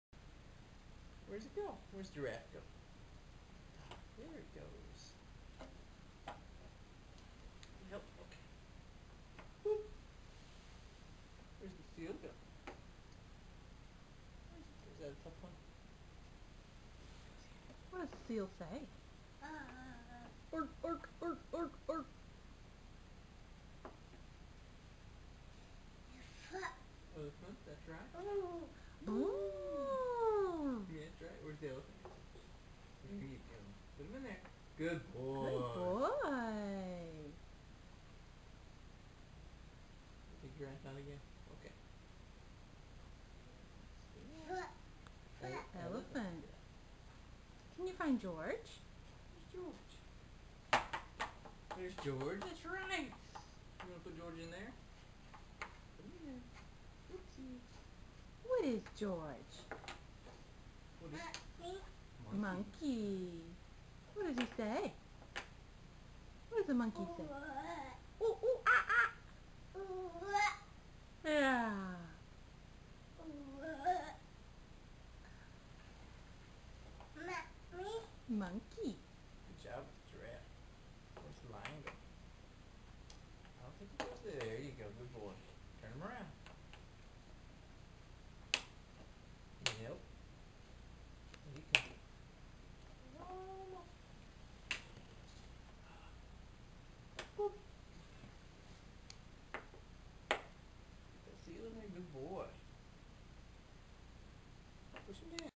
Playing with George puzzle.wmv
Elias playing with the Curious George puzzle that Aunt Jessica and Uncle Matt got him.